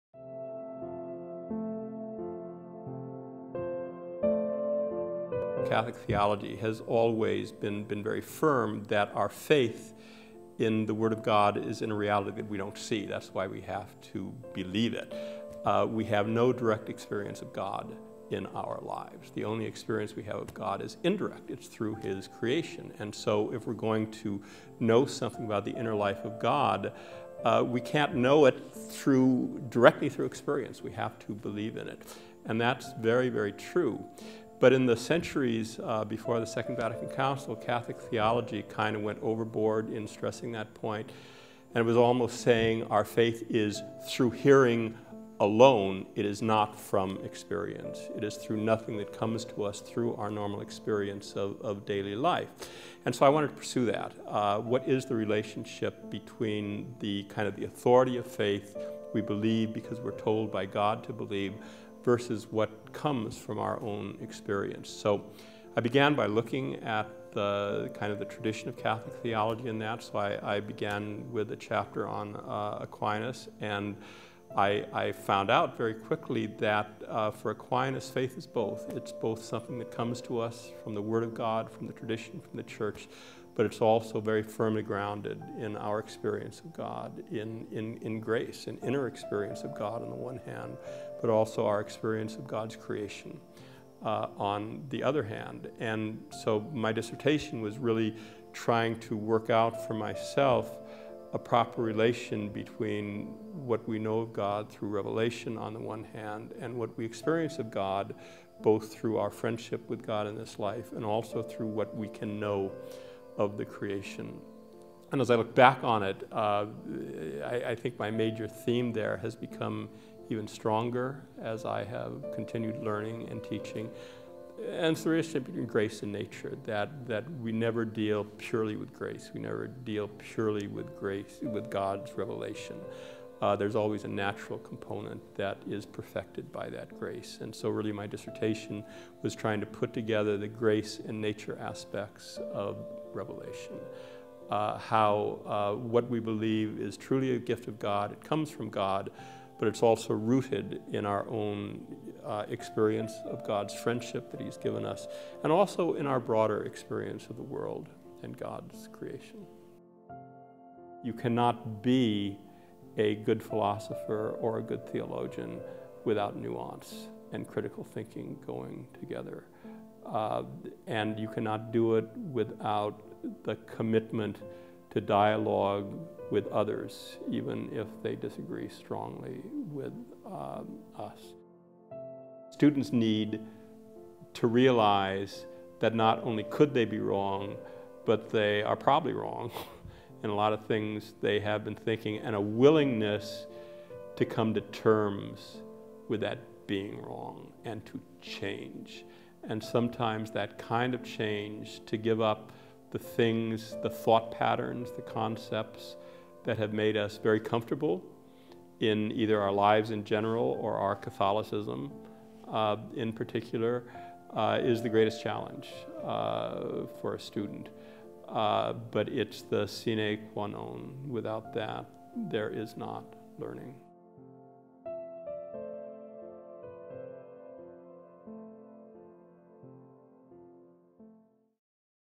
Ed Krasevac, OP, DSPT Professor of Theology
A short biographical introduction to Fr. Ed.